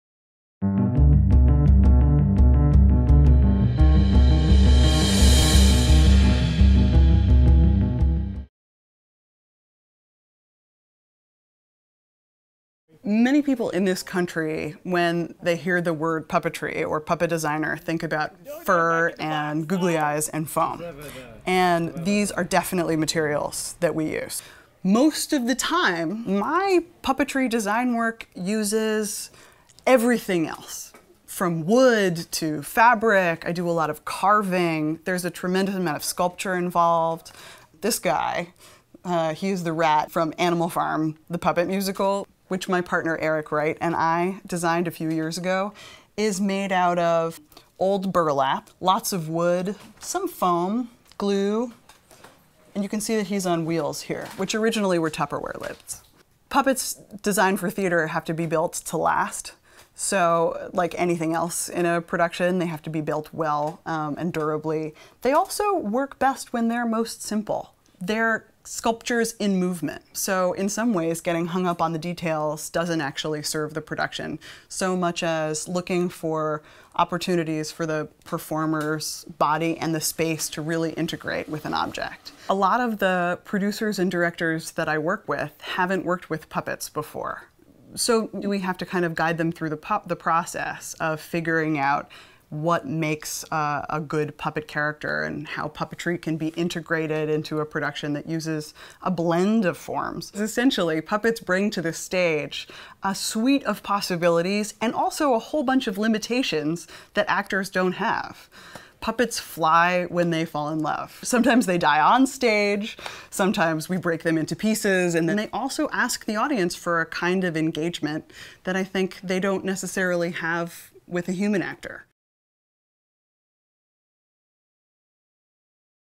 Classic Clips: Puppetry (2010)
Behind the scenes with Emily Decola, Puppet Designer.